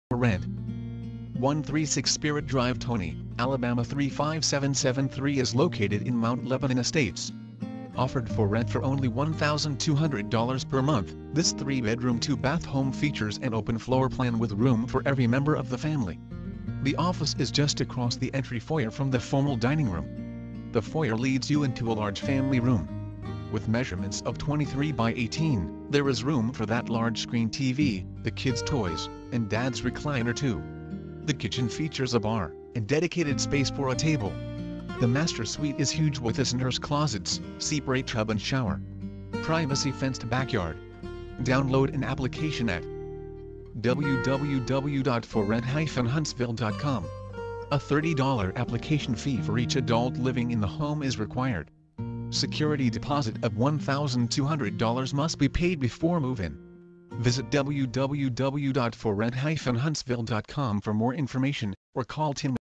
136 Spirit Dr Toney AL
For Rent - 1 3 6 Spirit Drive Toney, AL 35773 is located in Mount Lebanon Estates Offered For Rent for only $1,20000 per month, This 3 bedroom 2 bath home features an open floor plan with room for every member of the family Download an application A $3000 application for each adult living in the home is required Security Deposit of $1,20000 must be paid before date of occupancy Visit wwwForRent-Huntsvillecom for more information, or call Tim at to schedule a private viewing

Video uploaded on Sat 04 Jun, 2011
real estate Toney AL